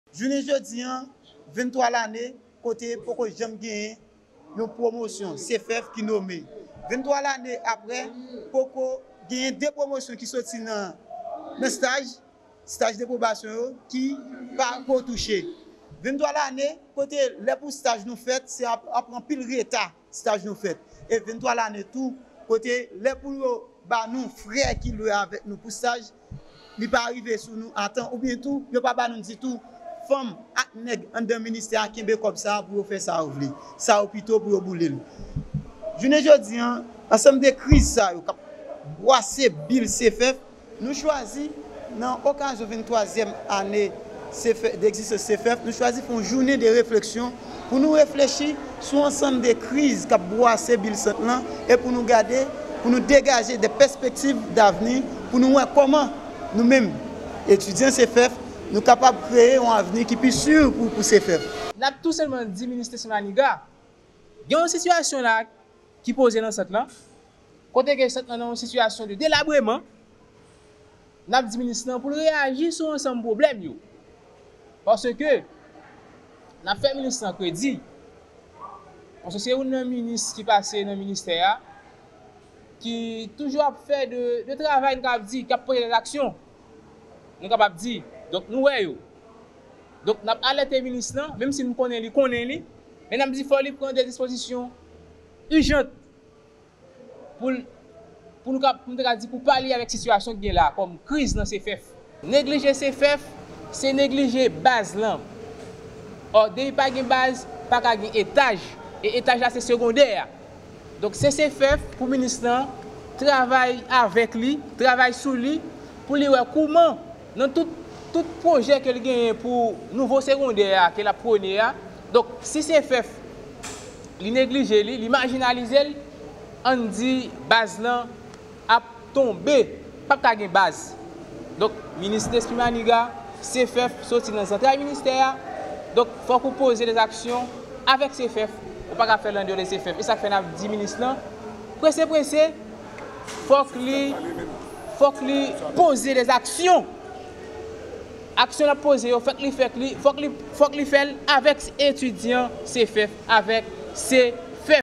Activités commémoratives du CEFEF
Pour célébrer le 23eme anniversaire du Centre de formation pour l'école fondamentale, des étudiants ont organisé ce vendredi 10 juin 2022, une journée de réflexion autour du thème : Un CEFEF pour toute Haïti, au local du Lycée Toussaint Louverture. Ont pris part à cette activité des étudiants et syndicalistes du secteur de l'éducation. Des étudiants du CEFEF, en saluant les efforts de Nesmy Manigat à la tête du Ministère de l'éducation nationale, invitent le ministre à accorder de l'importance à la formation des enseignants, notamment de résoudre la crise qui prévaut au CEFEF, s'il veut vraiment que ses diverses actions dans le système éducatif ne soient pas vaines
Nous vous proposons de suivre un extrait des déclarations des étudiants Frantz Dura et James Bellony, tous deux étudiants de la promotion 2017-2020 du CEFEF. Ils étaient au micro de Christian Aristide.